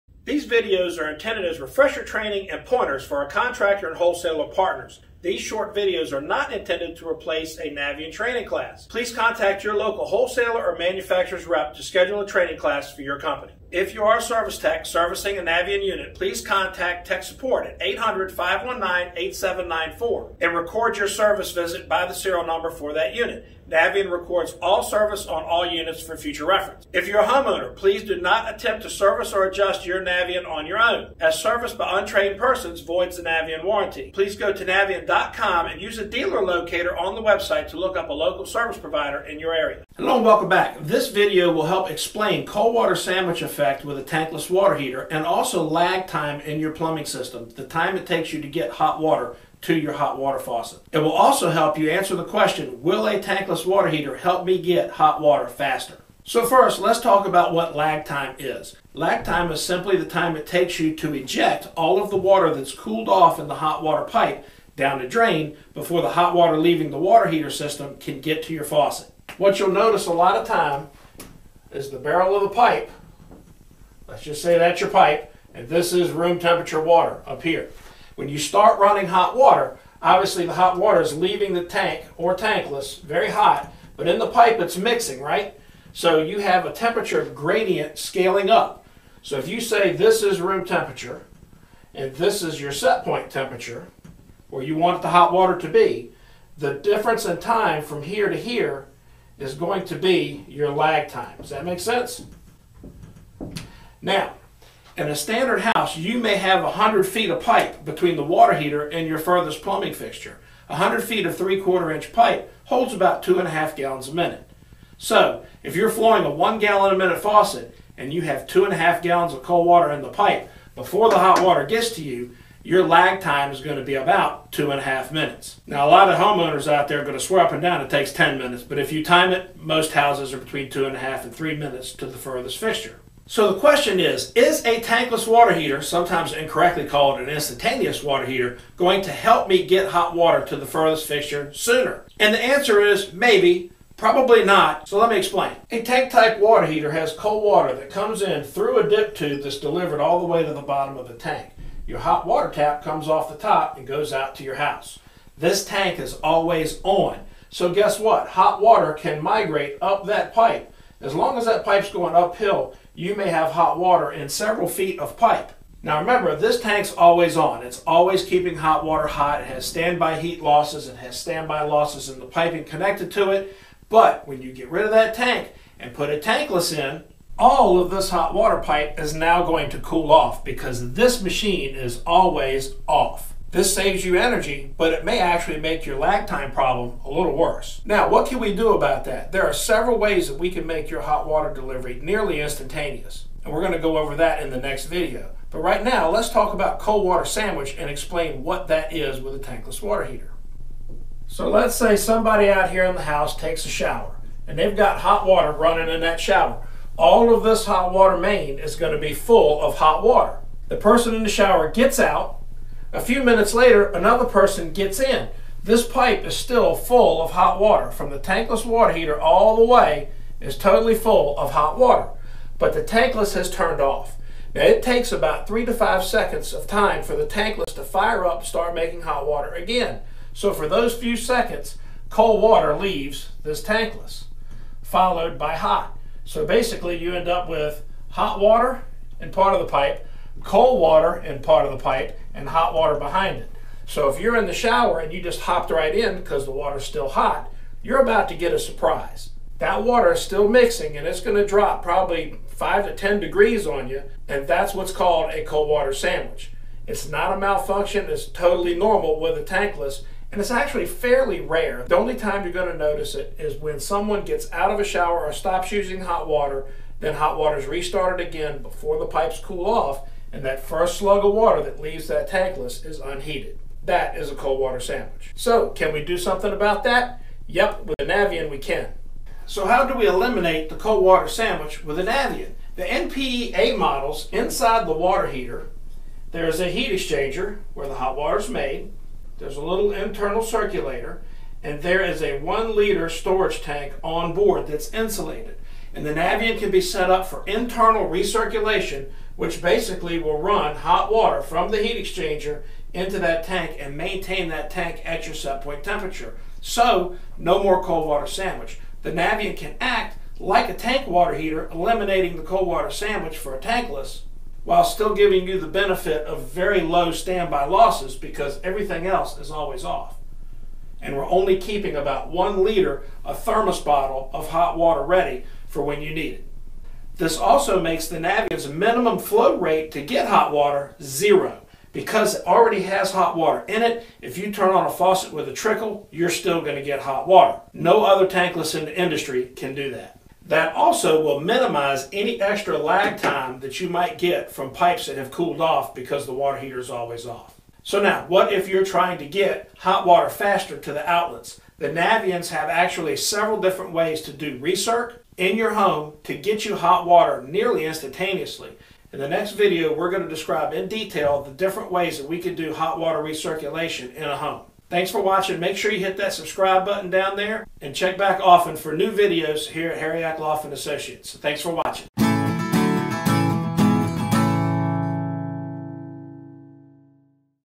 Lag Time and Cold Water Sandwich Explained
Tankless / Instantaneous water heaters, Lag time, cold water sandwich explained.  Will a tankless water heaters reduce your wait for hot water?  Navien can but first you have to understand what is happening in your hot water system.